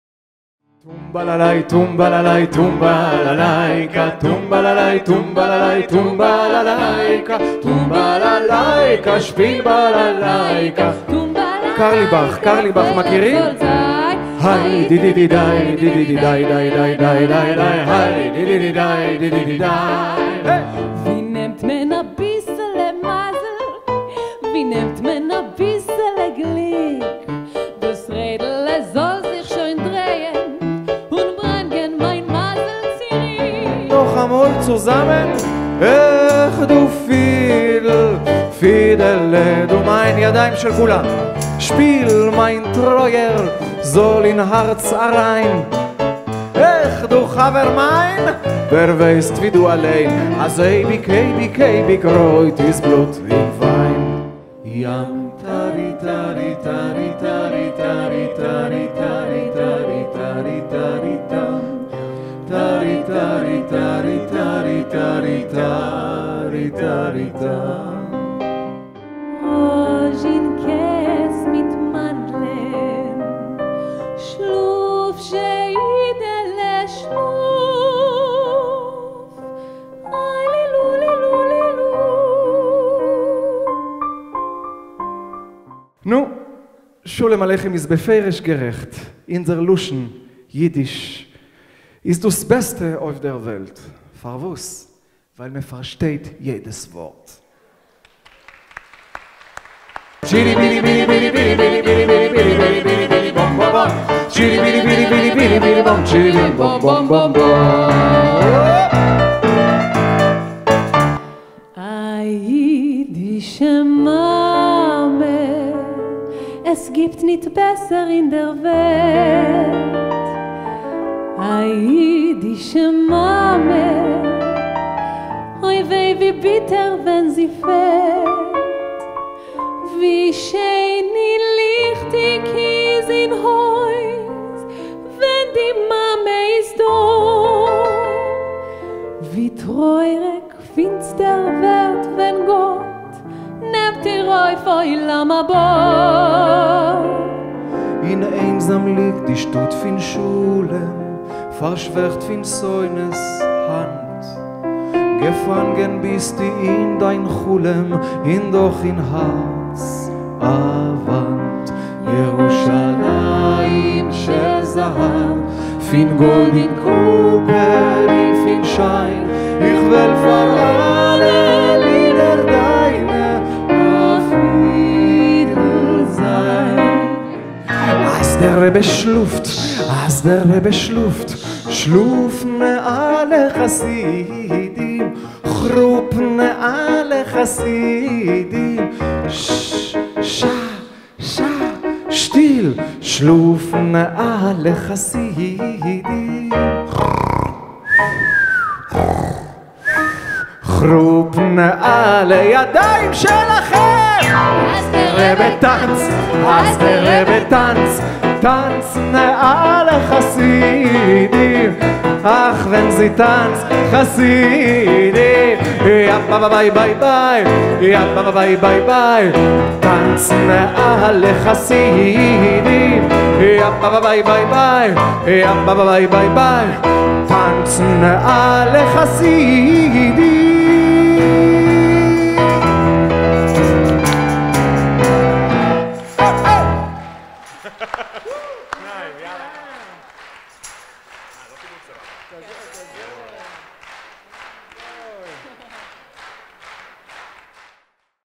SABRES - Yael Yekel & Yoni Eilat Yiddish Show 2020
SABRES: A Fresh, funny and moving live musical show by the young generation of Yiddish performers.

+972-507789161

Piano: Lior Ronen
Filmed by: Boaz Ben-Ezra
Edited by: Yael Yekel